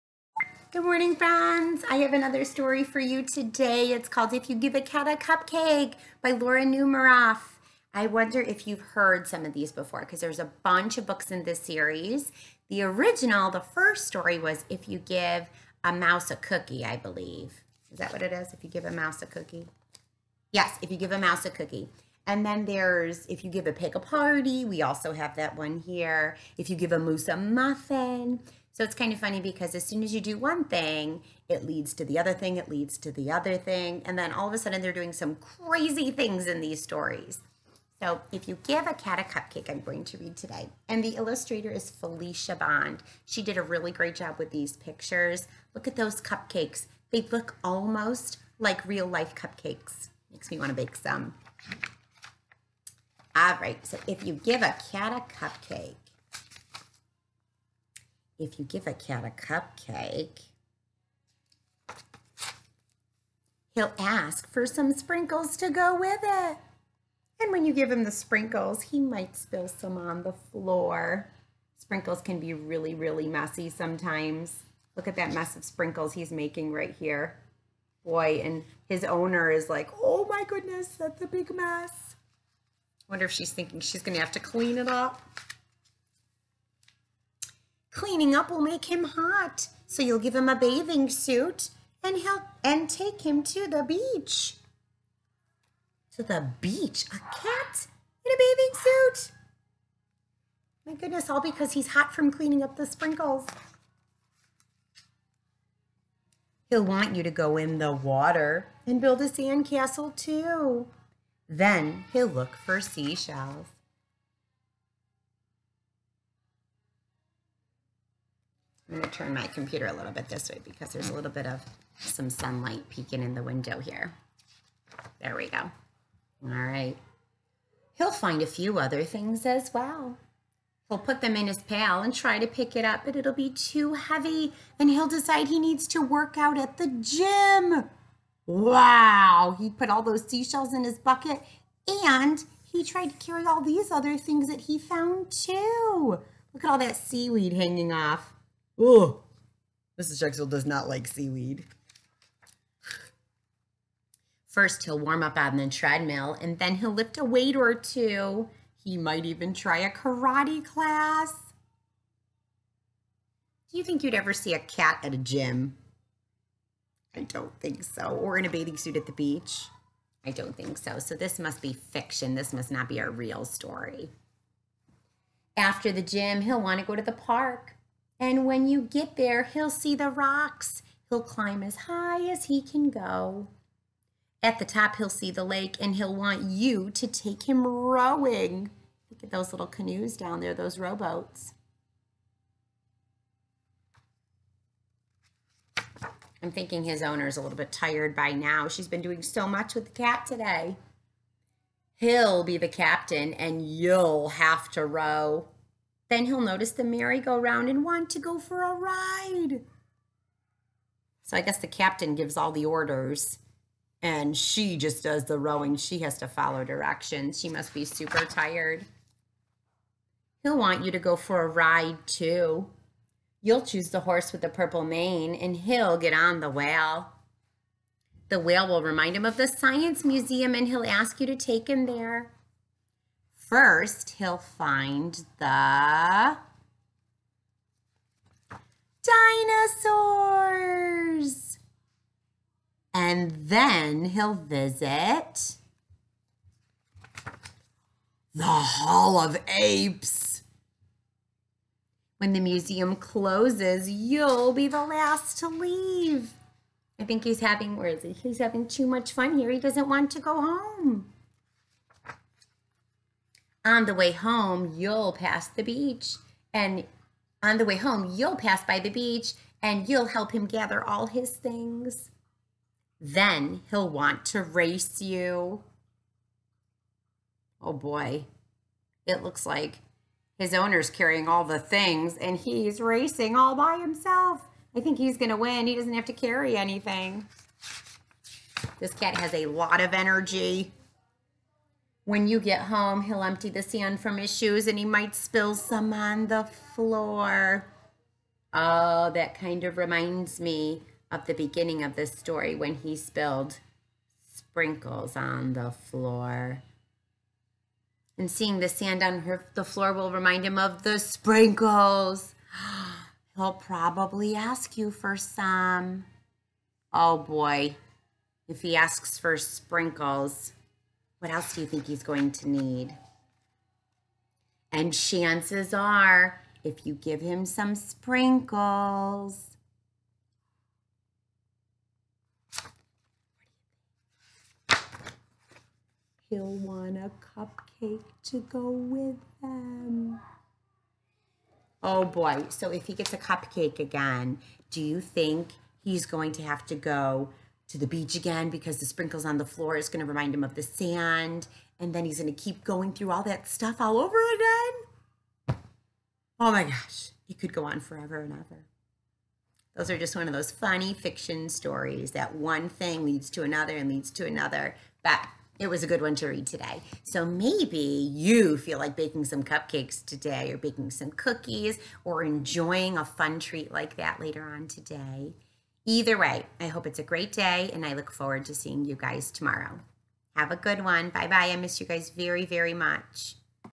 If You Give a at a Cupcake
Today I will read If You Give a Cat a Cupcake by Laura Numeroff